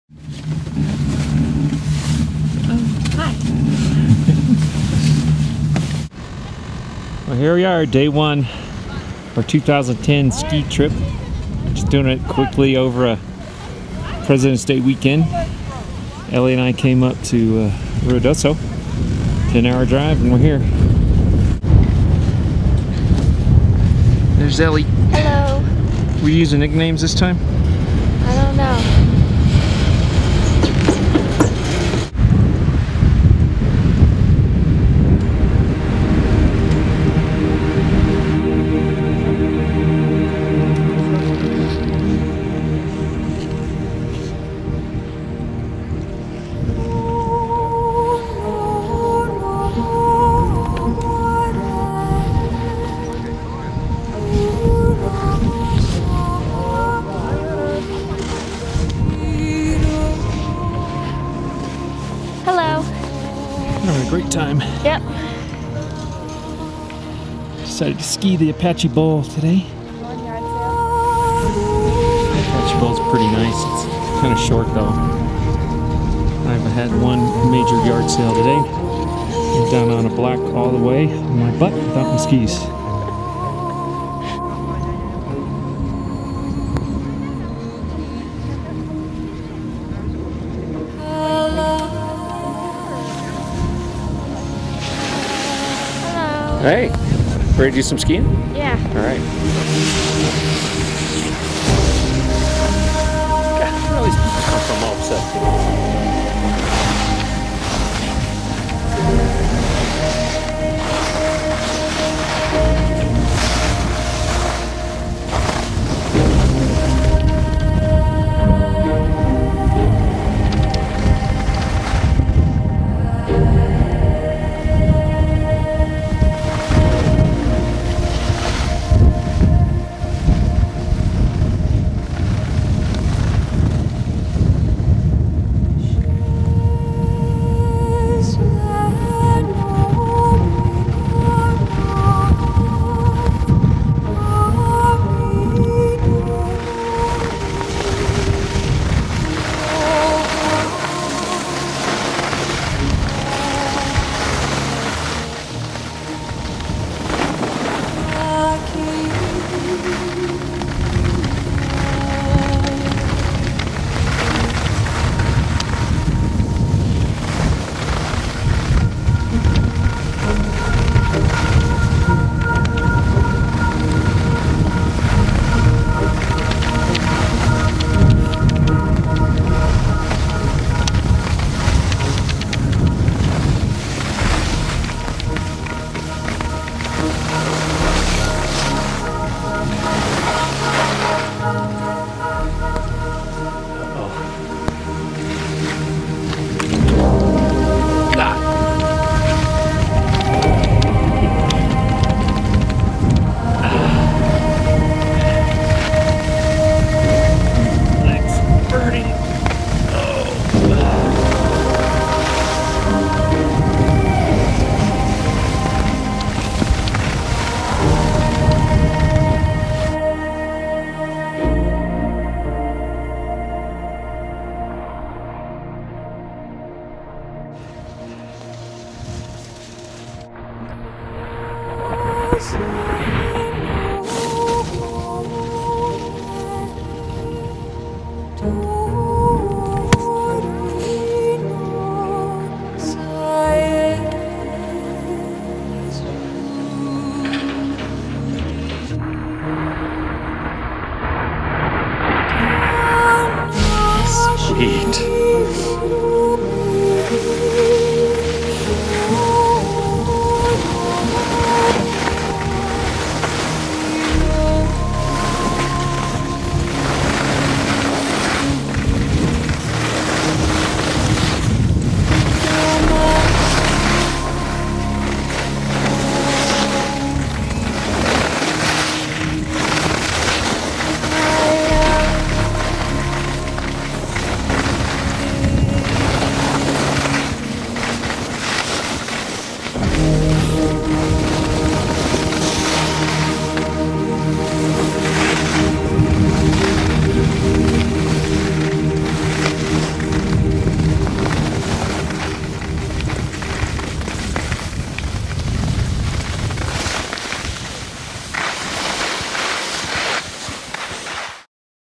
Ski Apache at its Best
The HD Avatar journeyed to Ruidoso NM over the President's Day holiday weekend for some extreme skiing. I don't think there has ever been better snow at Ski Apache. Check out this amazing footage.